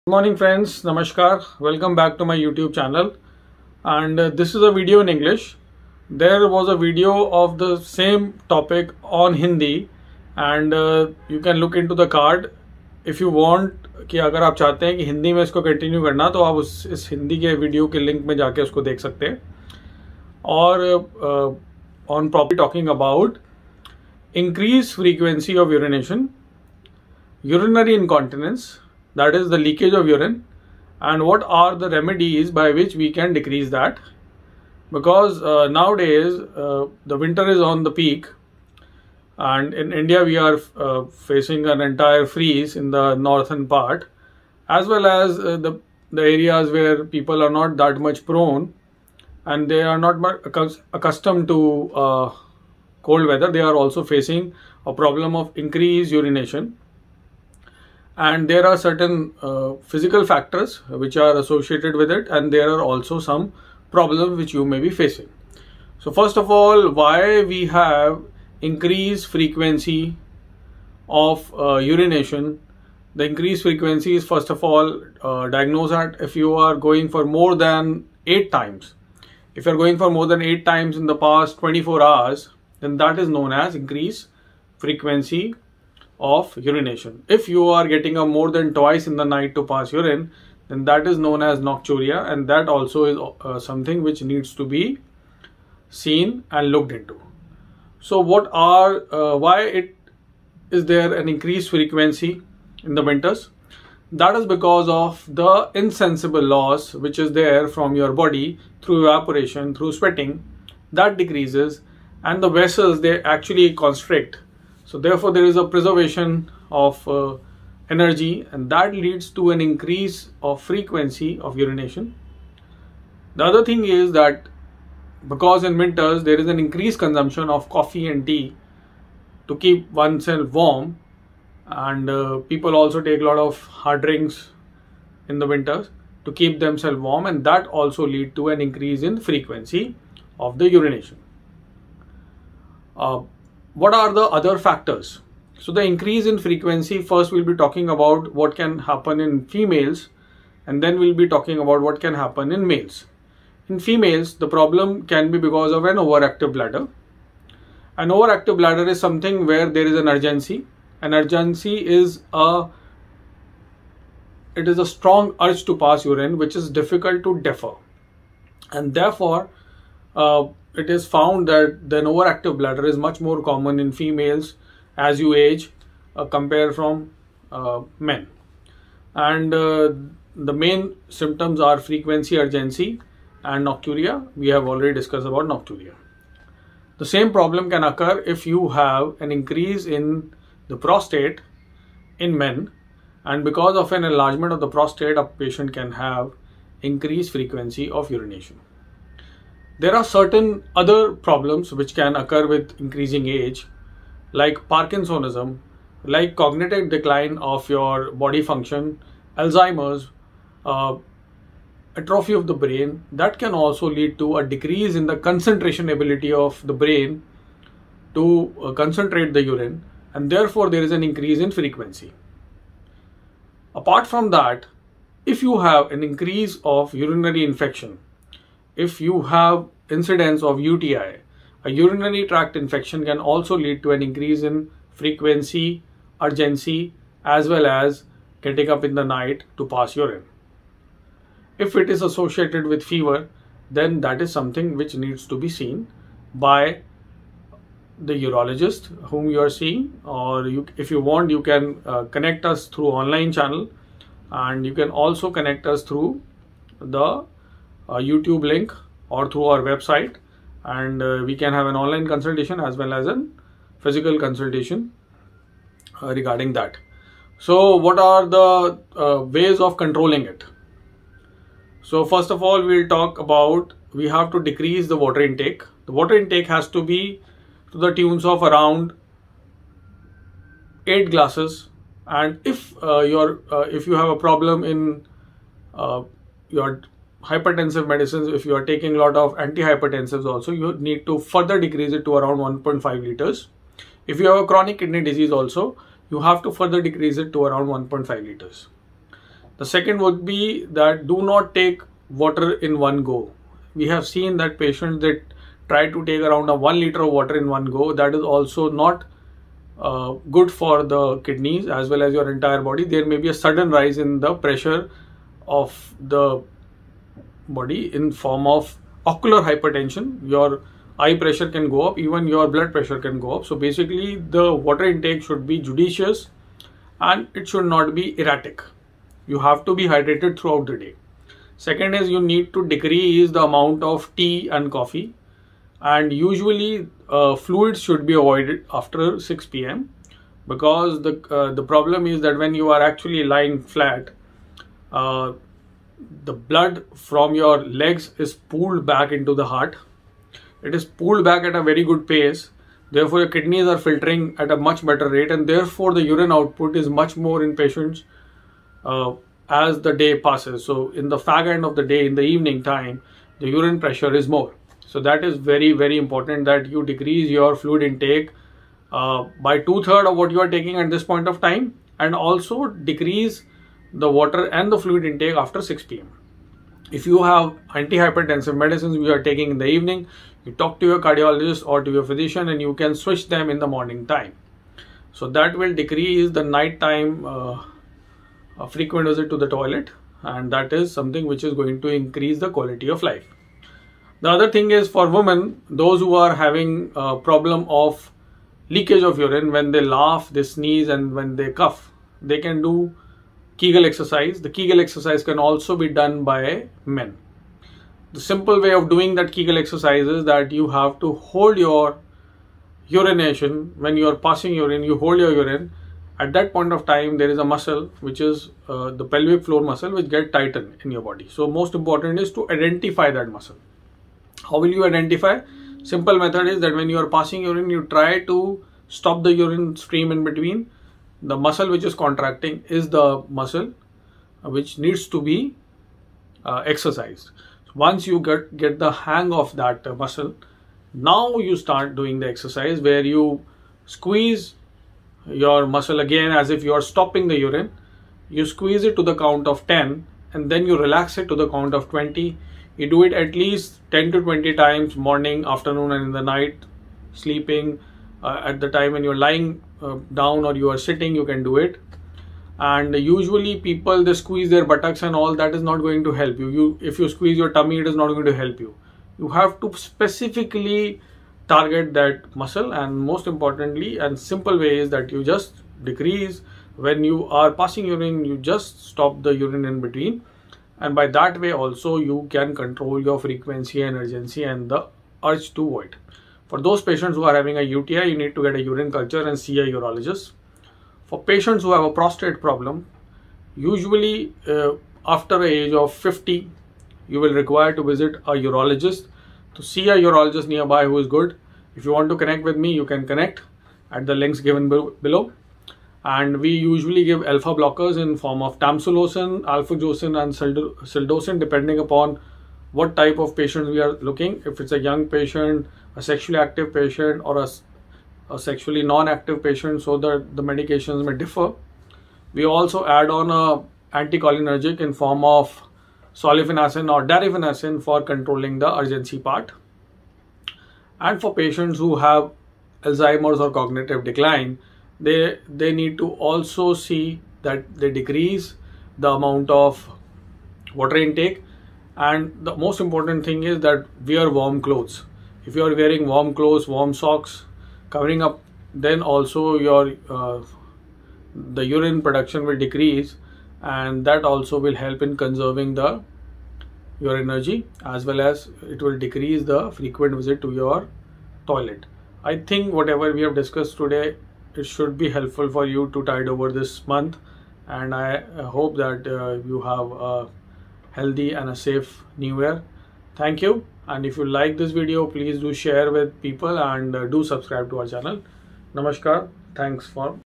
(2022) Frequent Urination in Winters Causes and Treatment (English)
This video tells you about a common problem of frequent urination in winters and how to get better